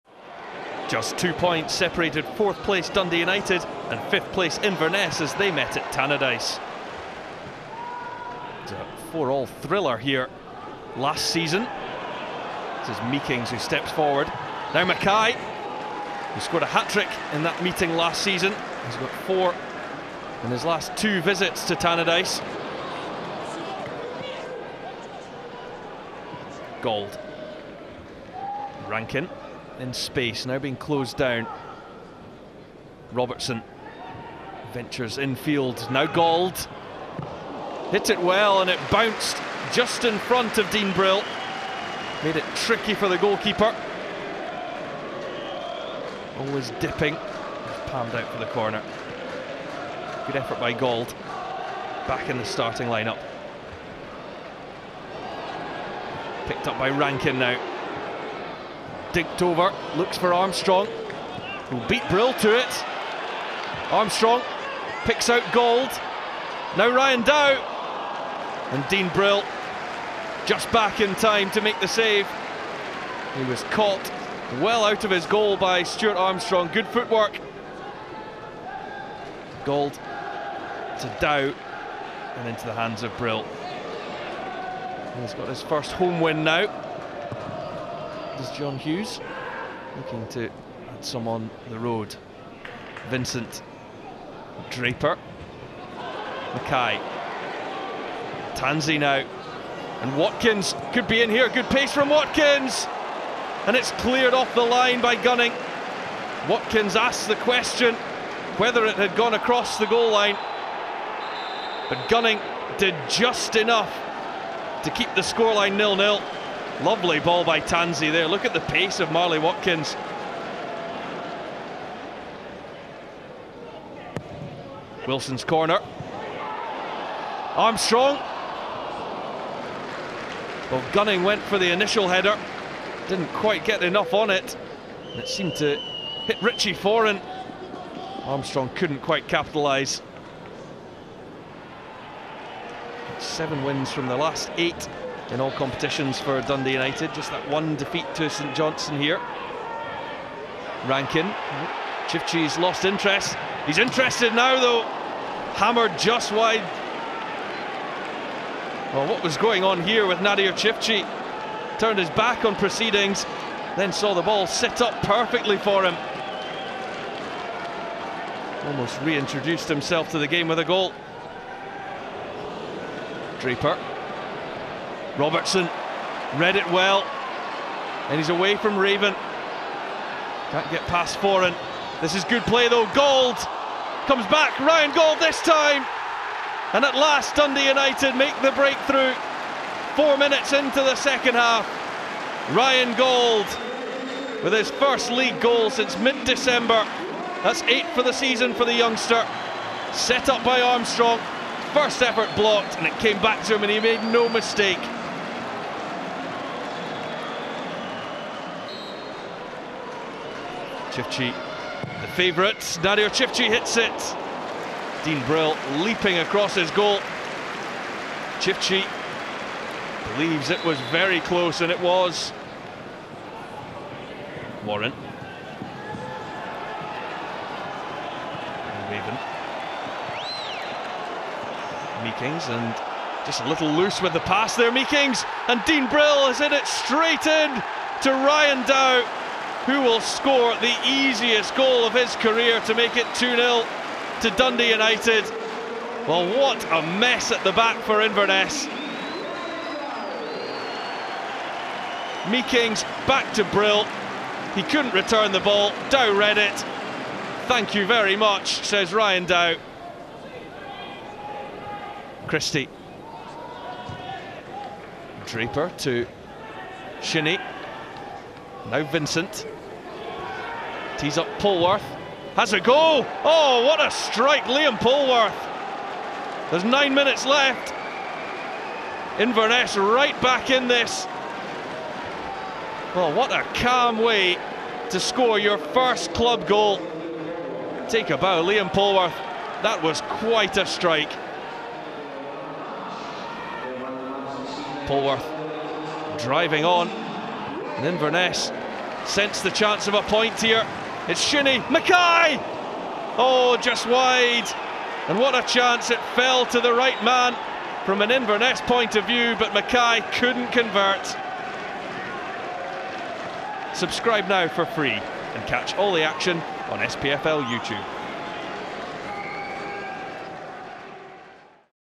Unbelievable blooper and stunning goal as Utd beat Caley
Dean Brill's embarrassing blooper helped Dundee United to a 2-1 win over Inverness CT at Tannadice in the SPFL Scottish Premiership, despite Liam Polwarth registering a goal of the season contender at the other end. Ryan Dow was the beneficiary of the error while Ryan Gauld's opening goal proved the difference between the sides as United kept their hopes of a second place finish alive.

Dundee United v Inverness Caledonian Thistle | 26/03/2014 ko 19:45:00